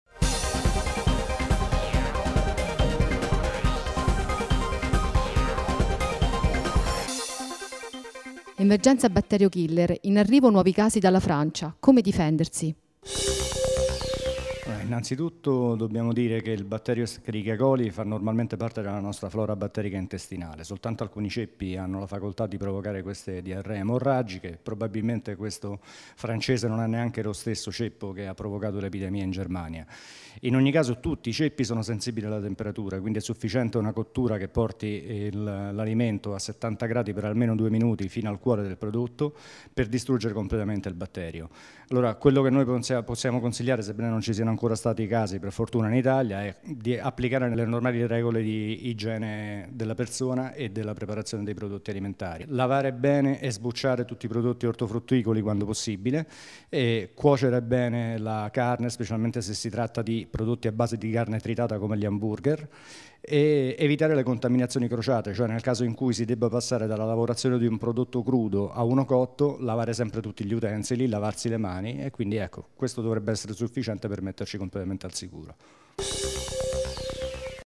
Emergenza Batterio Killer: in arrivo nuovi casi dalla Francia
Come difendersi?

R: Claudio Lucchetta - Resp. Sicurezza alimenti ADICONSUM